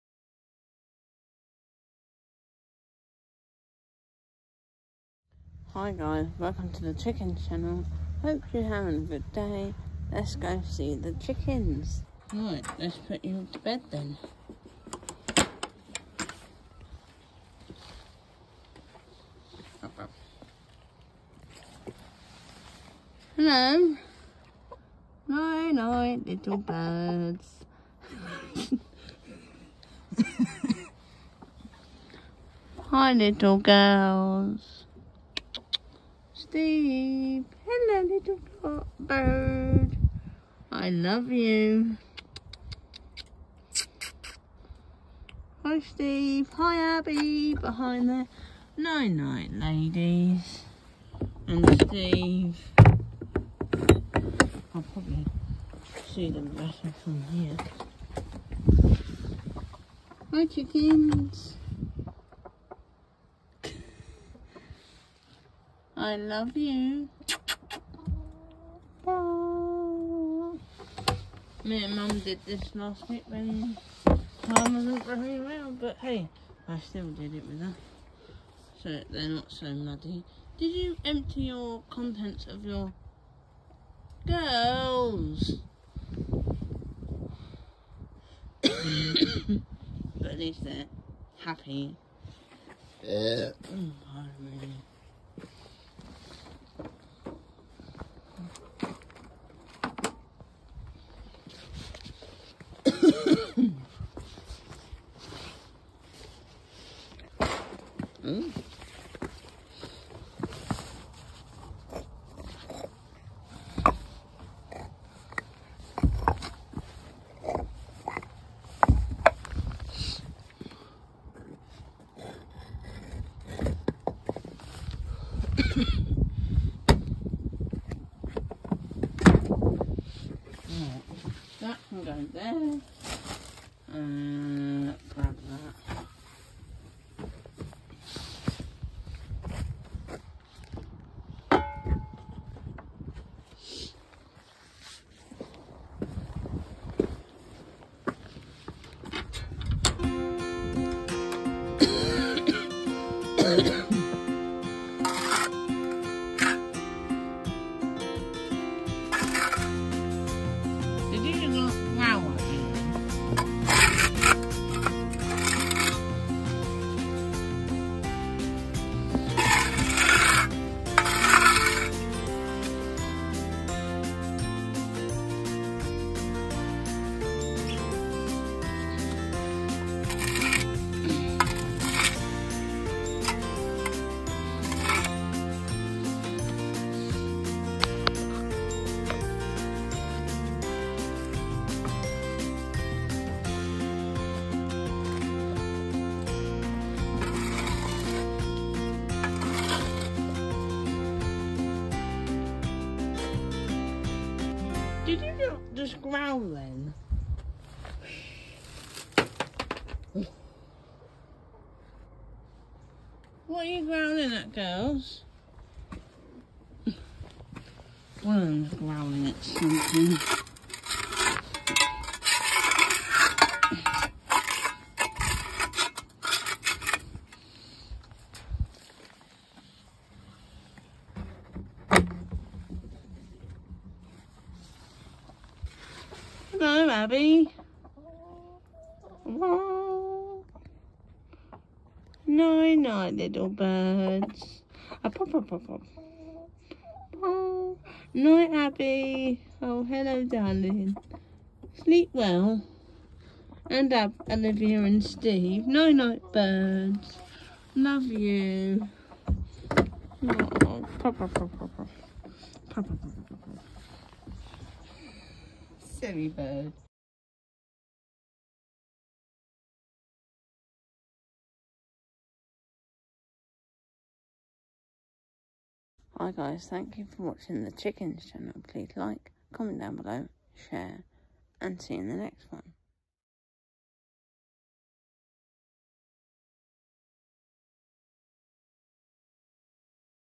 bedtime
putting the chickens to bed and I love them so much because they are so good for me, they are kind and make a nice breakfast from the eggs they lay
I am kind to them